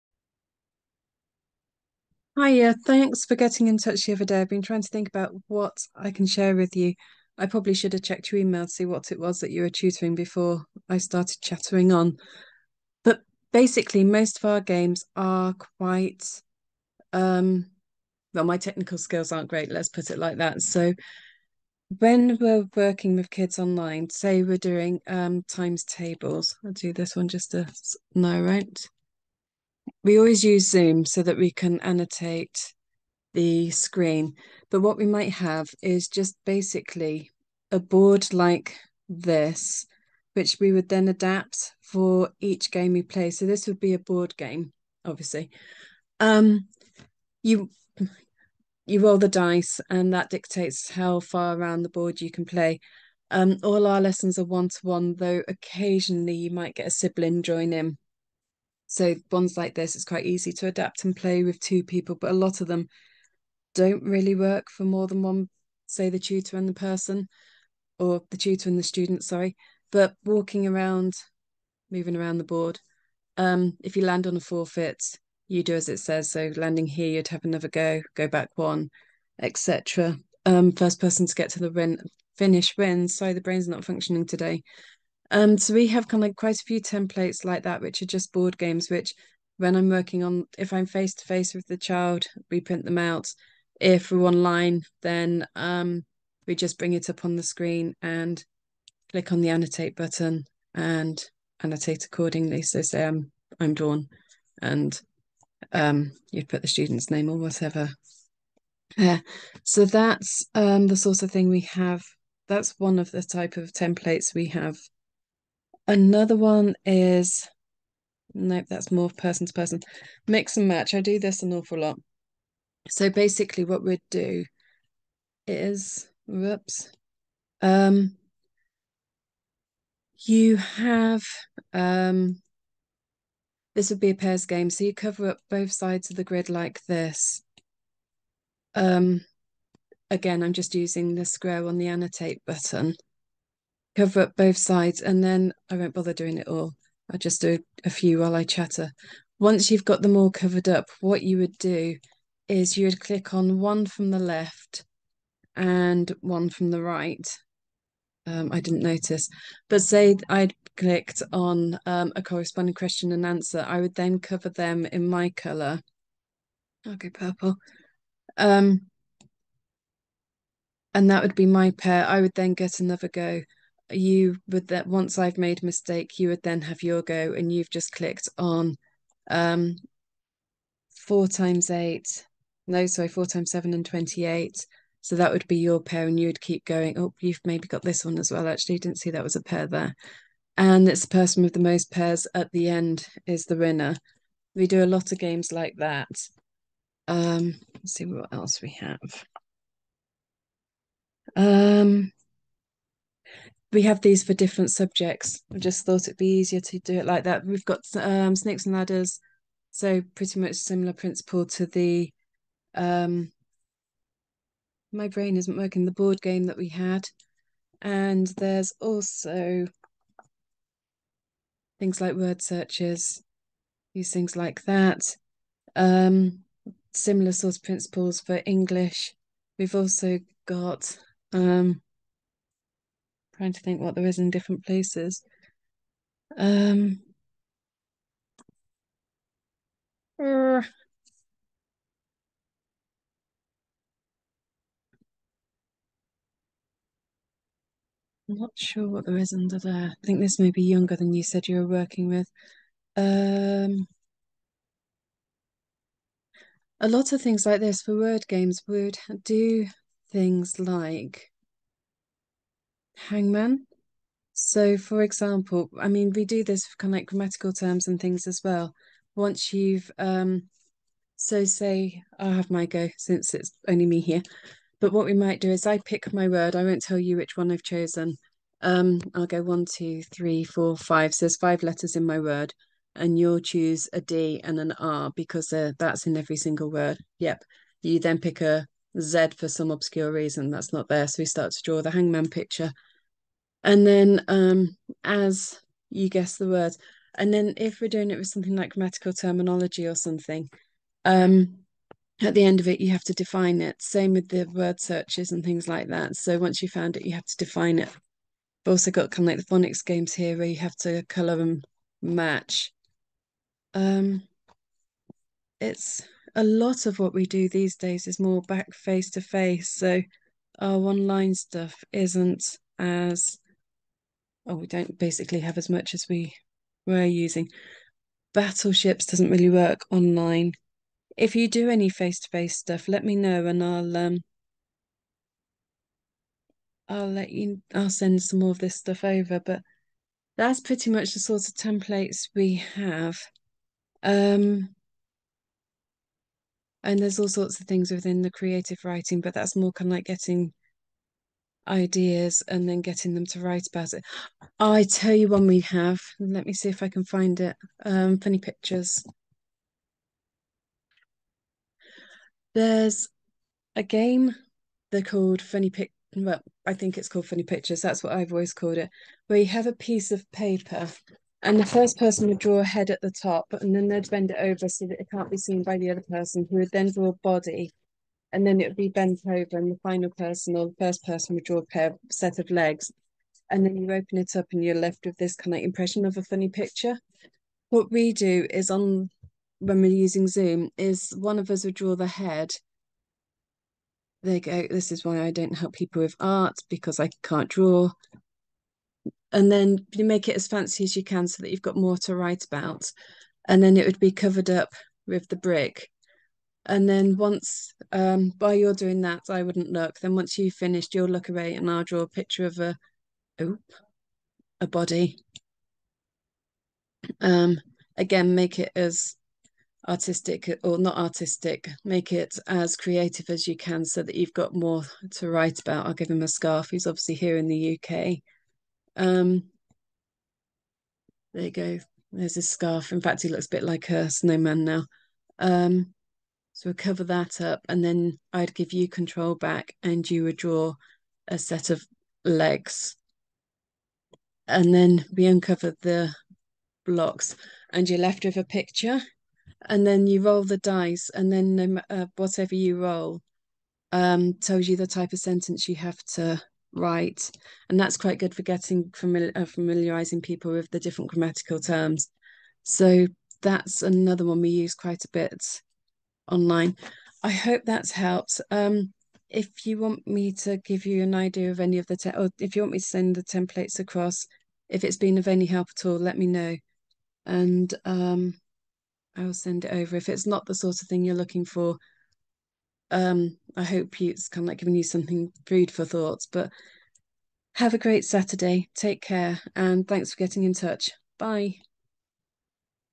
Some of the online resources we use at Clara James
Good afternoon,

I am a fellow tutor and was wondering if you offered online games/resources/ideas for tutoring over zoom.  I can't afford a franchise, but I may be able to purchase some materials.   Would you have any content that provides directions on how to set up online games that are interactive where the student can also move around the pieces/letters/numbers on their screen.  Or if you have templates for the games or the actual games themselves, that could be very helpful as well.

After a couple of clicks we established that she is in the US, I'm in the UK so I said I'd send her a video of the types of resources we use online.

If it's of any interest to you, this is the link I sent her. I hope it's of help